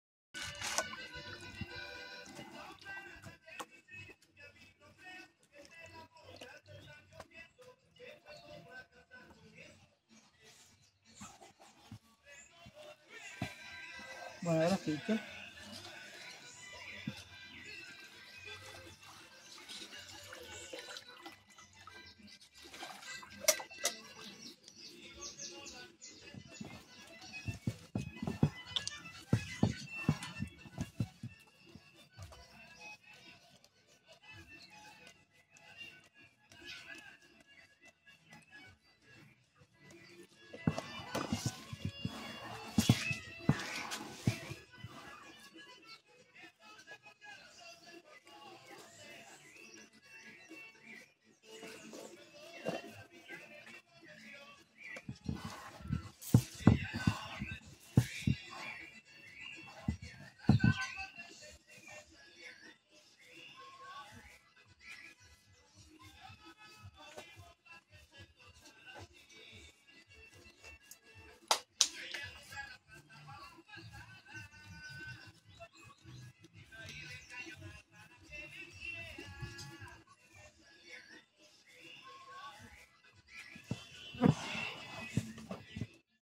Bolas de Queso
El queso es el producto más importante de la industria láctea en cuanto a consumo y producción mundial.
No hay lugar donde se produzca leche que no tenga una variedad interesante de quesos.
¿Con qué recomiendas comer las bolas de Queso?